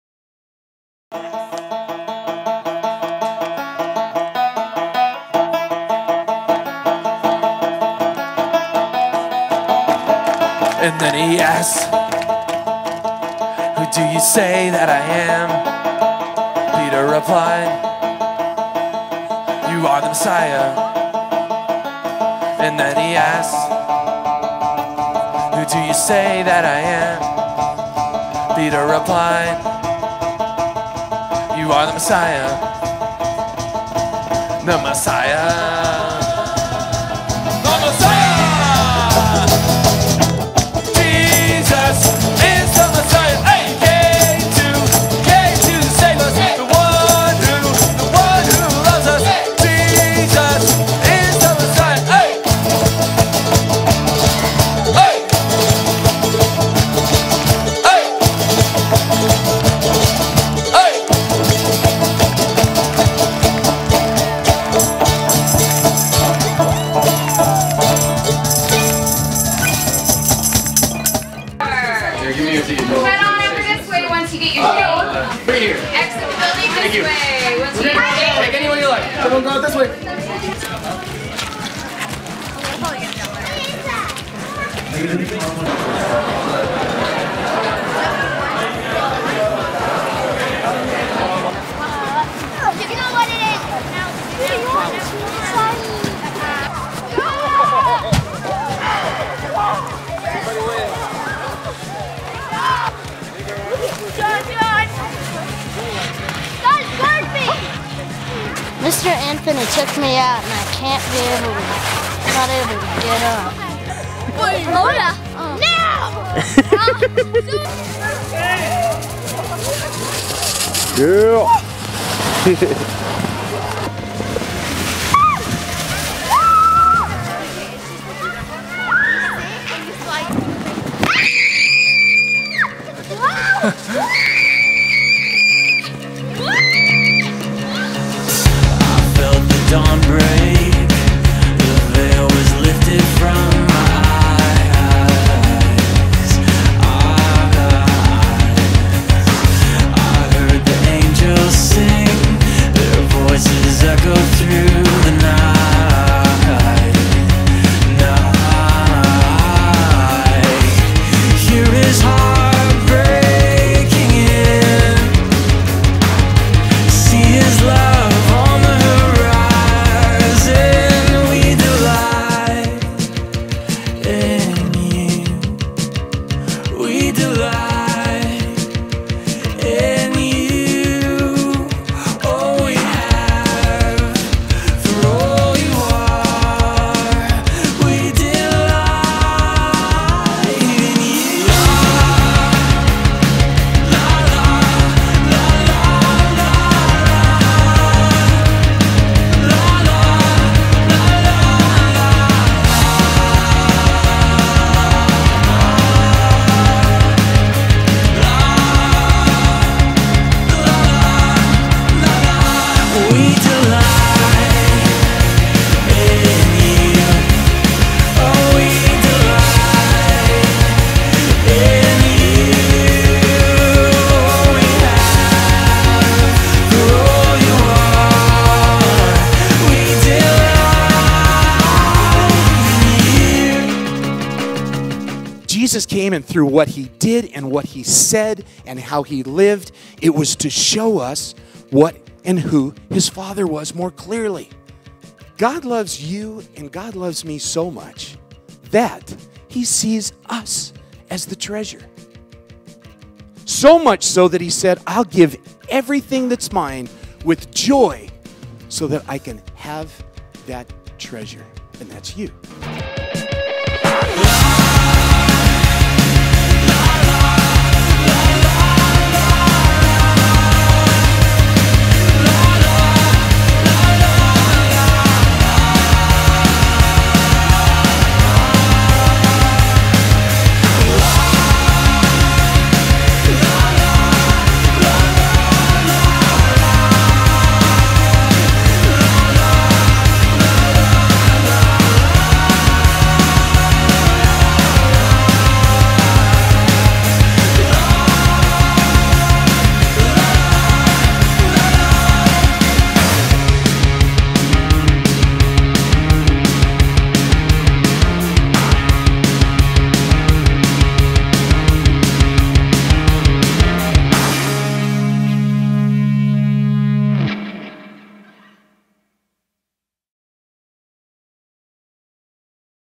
February 22-24 // Junior Winter Camp 2013
What an amazing time this weekend! Thanks for all the campers and counselors for coming up and helping make this time special!